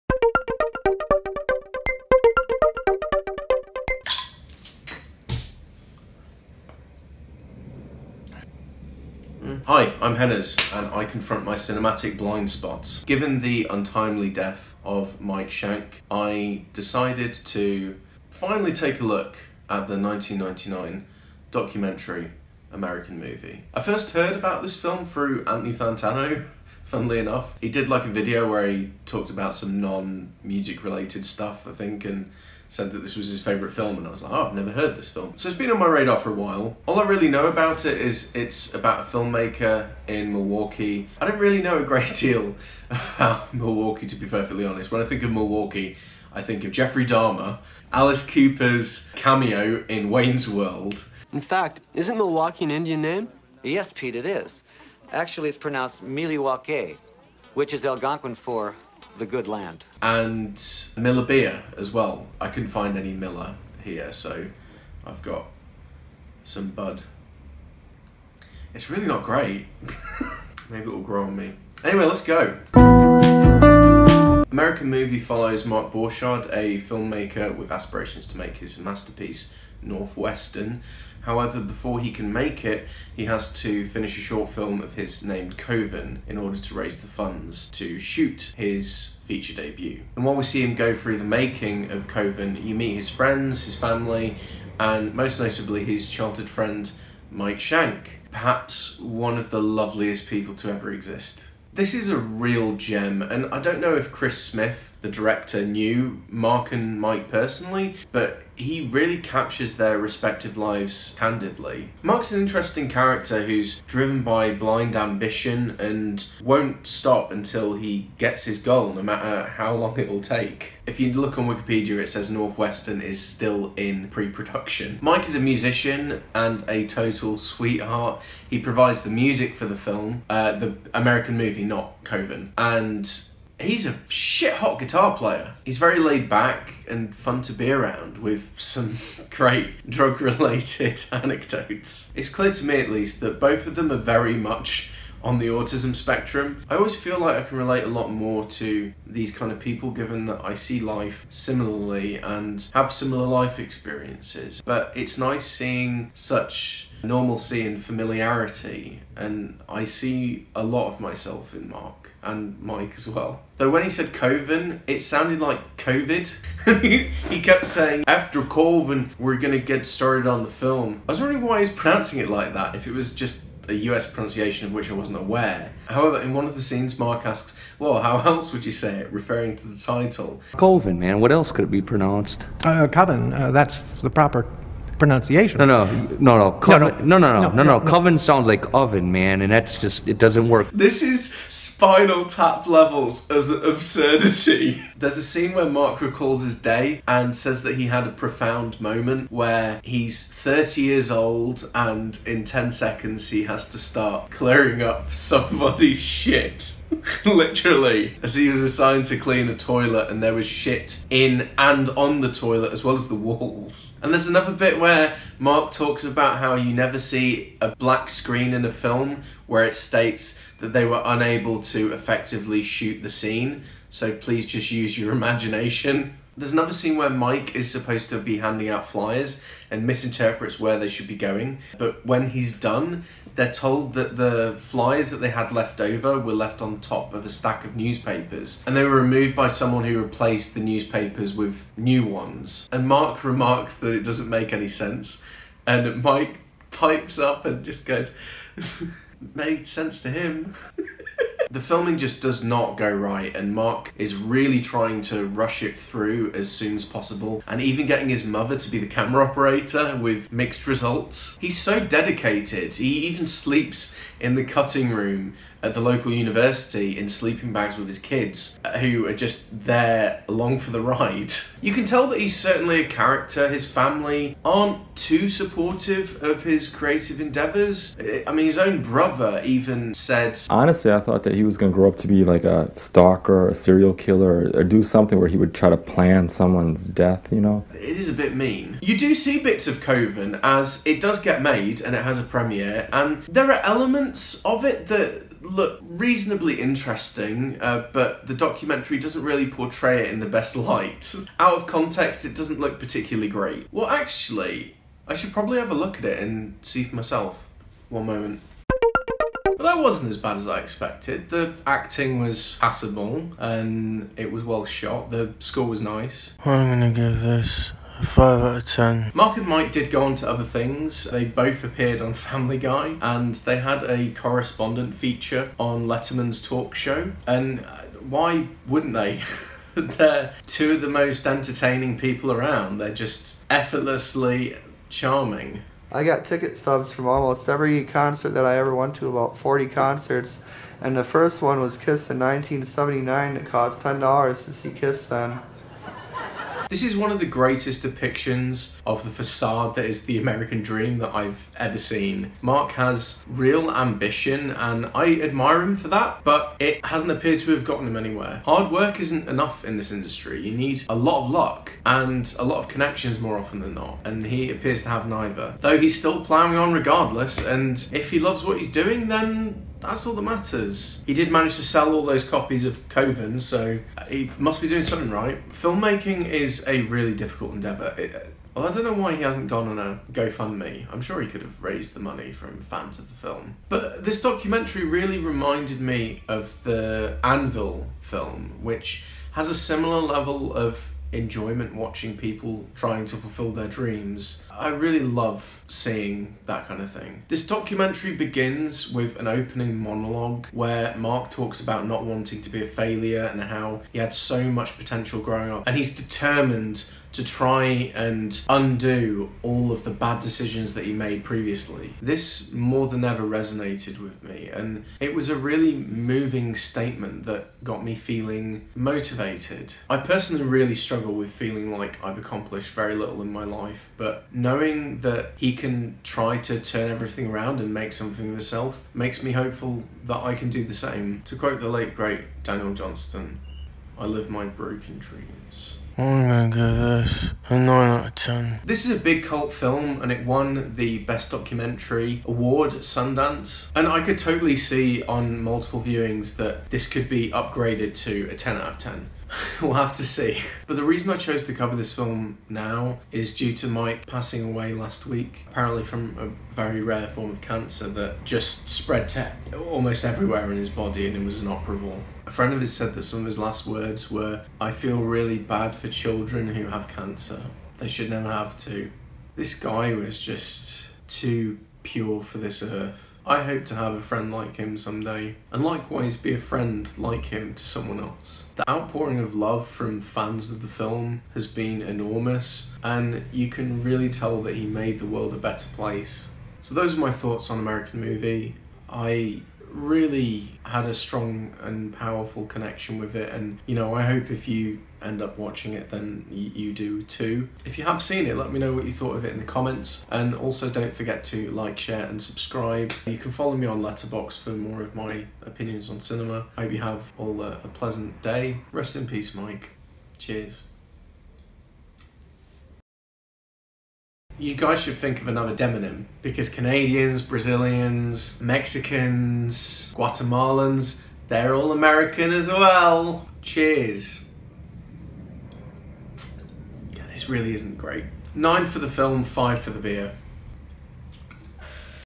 American Movie (1999) - Film Review
Given the tragic passing of Mike Schank, it's about time I finally watch American Movie. A documentary brimming with charm and drive.